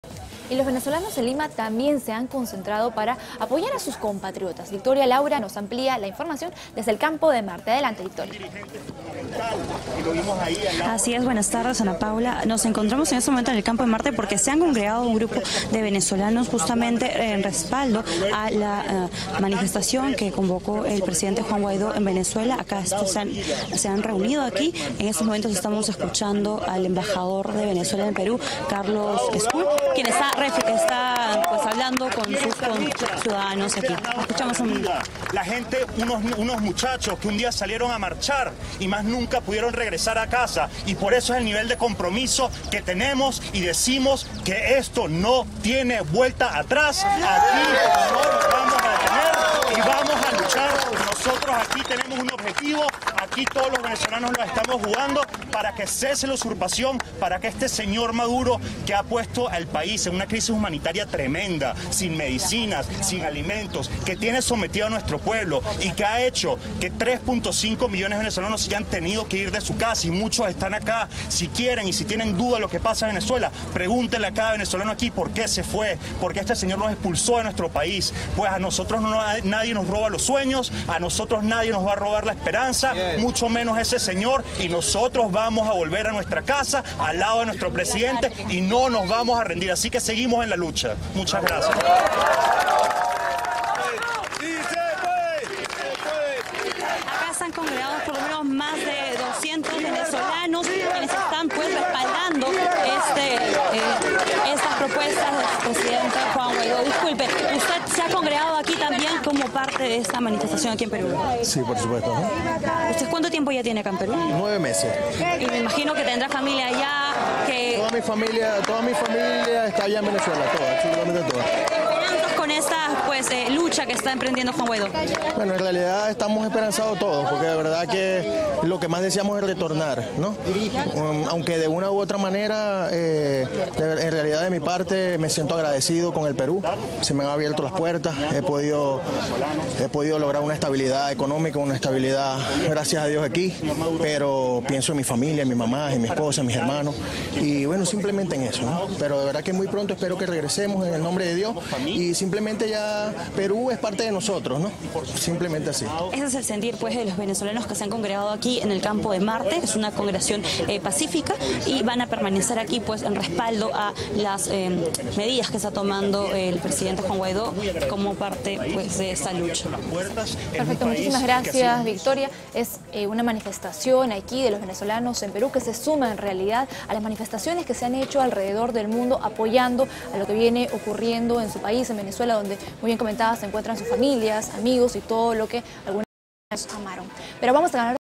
"Maduro hizo que 3.5 millones de venezolanos dejen sus casas", dice Carlos Scull | 90 Digital
90Digital | El embajador venezolano en Perú nombrado por Juan Guaidó llegó al Campo de Marte, donde participó de una congregación en apoyo a la Operación Libertad.